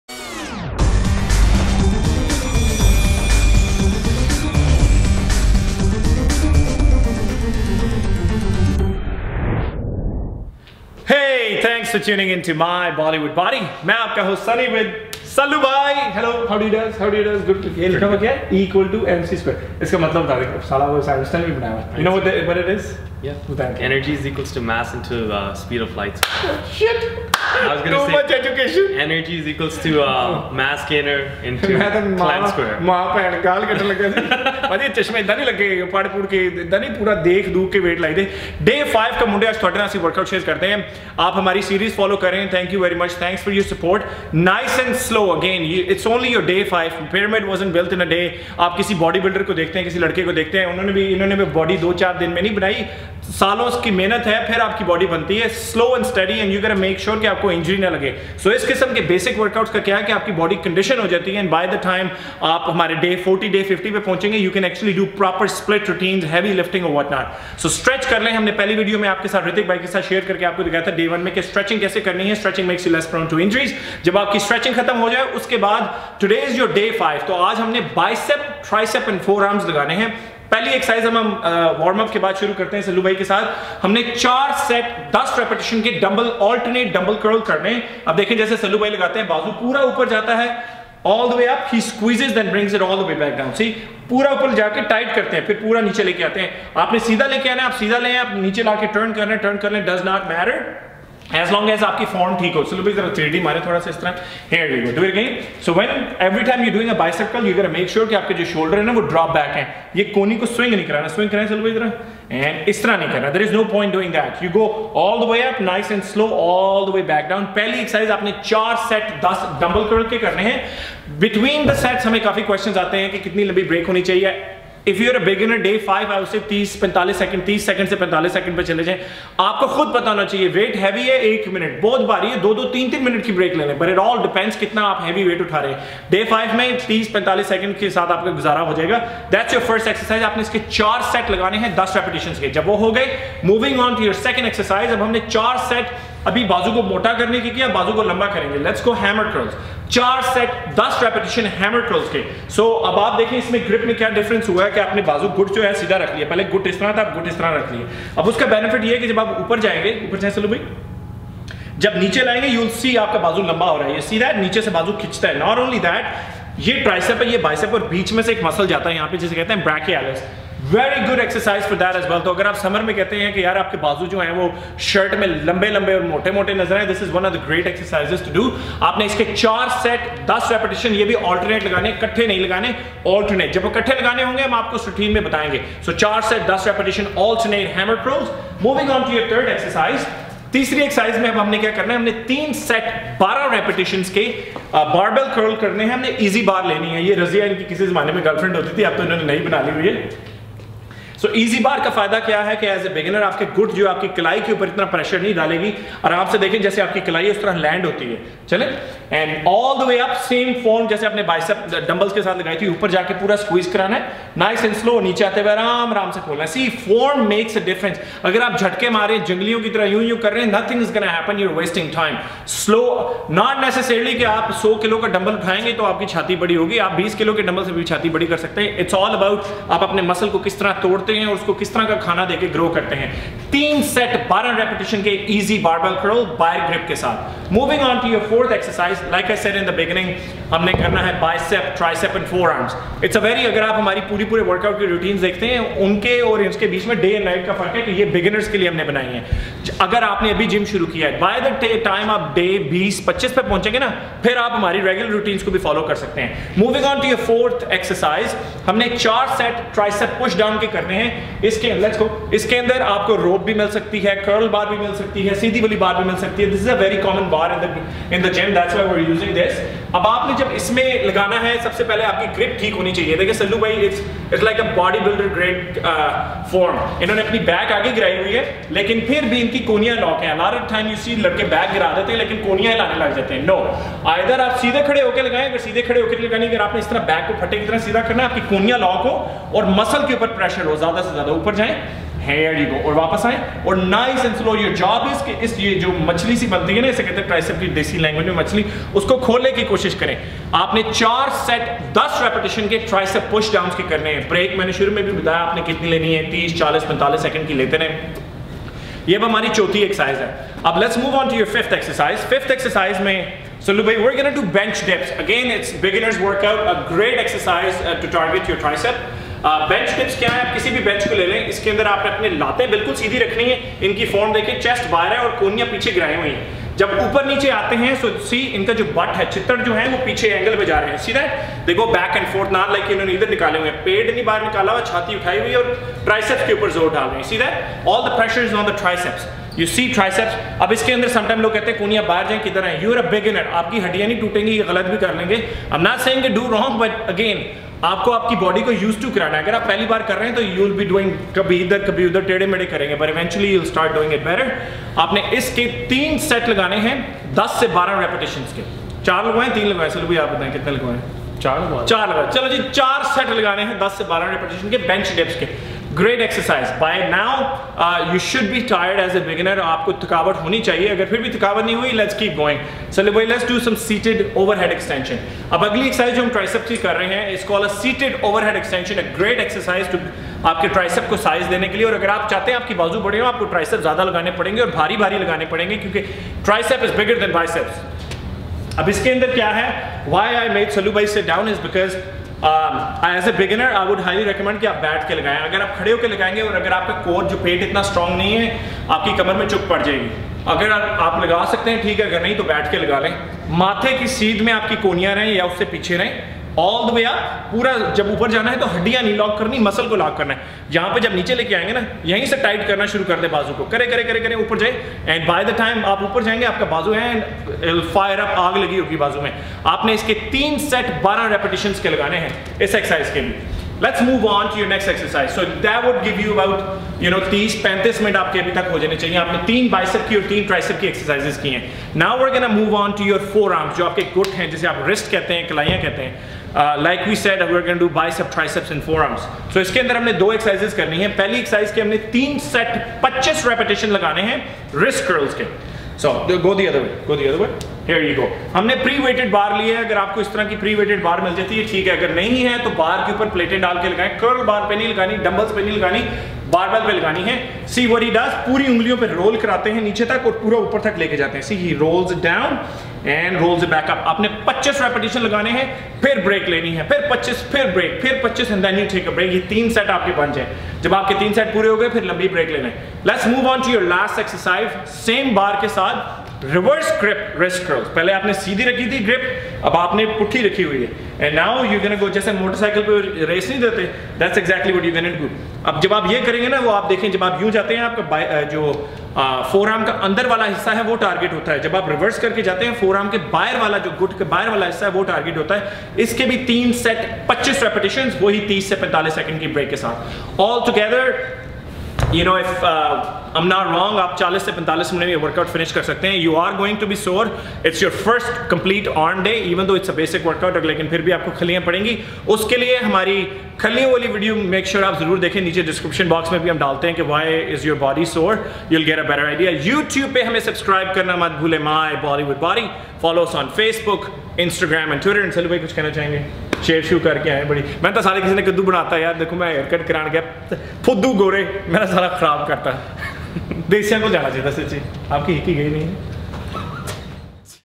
| DAY 5 | Bicep / Tricep & Forearms Workout for BEGINNERS! (Hindi / Punjabi)
For high quality protein powders and supplements I highly recommend MY PROTEIN

Now you can Train with Sunny!

As part of the beginners bodybuilding series we will be doing bicep and tricep as well as forearms workout. Make sure you stretch. Watch our first video for stretching or watch our stretching video.

Following is what we performed in this routine:
Alternate Dumbbell Curl
4 Sets of 10 Reps with 30-45 sec break

Alternate Hammer Curl
4 Sets of 10 Reps with 30-45 sec break

Barbell Curl (Ez Bar)
3 Sets of 12 Reps with 30-45 sec break

Tricep Push Down
4 Sets of 10 Reps with 30-45 sec break

Bench Dips
4 Sets of 10 Reps with 30-45 sec break

Seated Tricep Overhead Extension
3 Sets of 12 Reps with 30-45 sec break

Wrist Curls (Barbell)
3 Sets of 25 Reps with 30-45 sec break

Reverse Wrist Curl (Barbell)
3 Sets of 25 Reps with 30-45 sec break

If feeling SORE due to exercise!